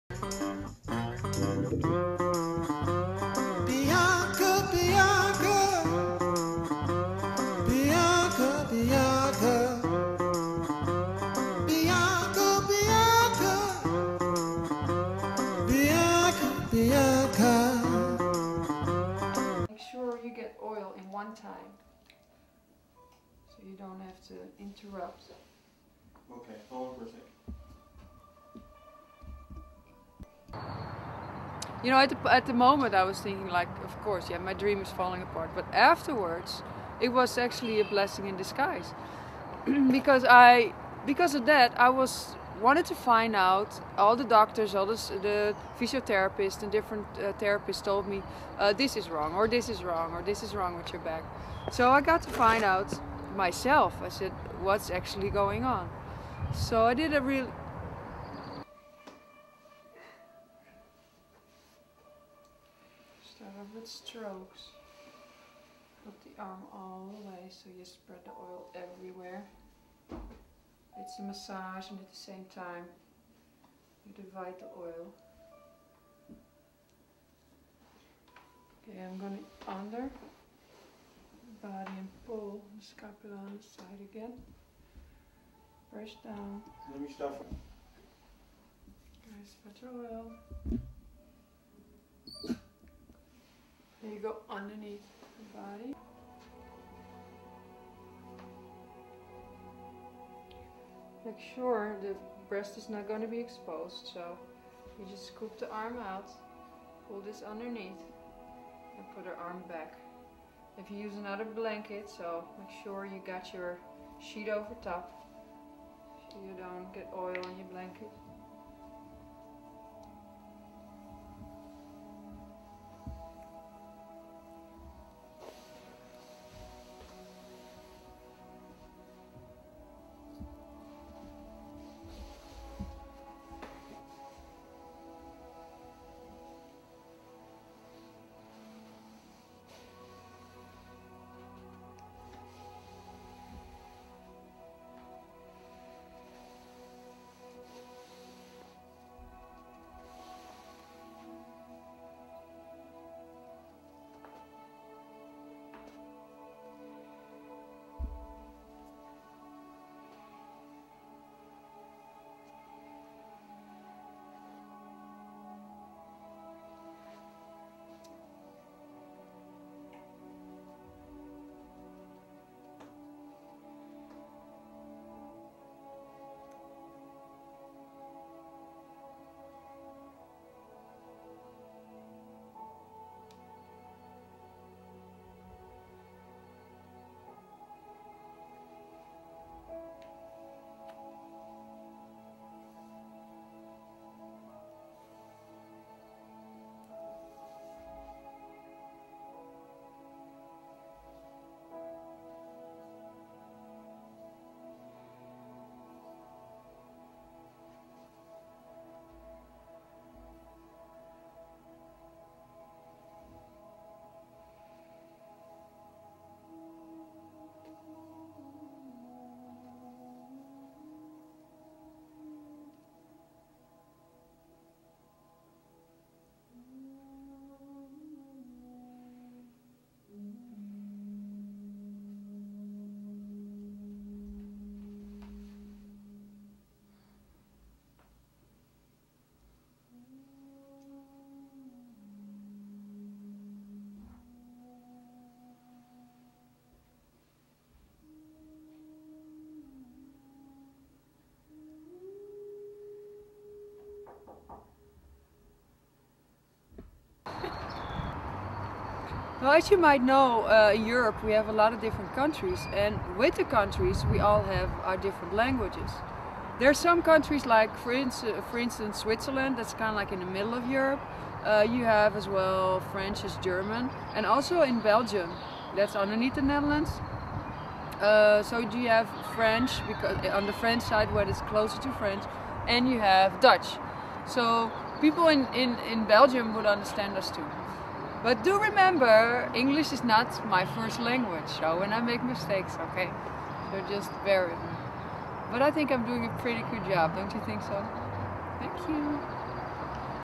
Massage arm
Bianca van Berkel; European Health & Wellness Coach.
Teaching massage: arm & shoulder & hand
How I got into massage after my back injury during my dance academy in Amsterdam, the Netherlands 15+ years ago.
Thanks to model Sunshine
Information about Europe.
Massage for adults and children

Massage practice in Minneapolis, MN, USA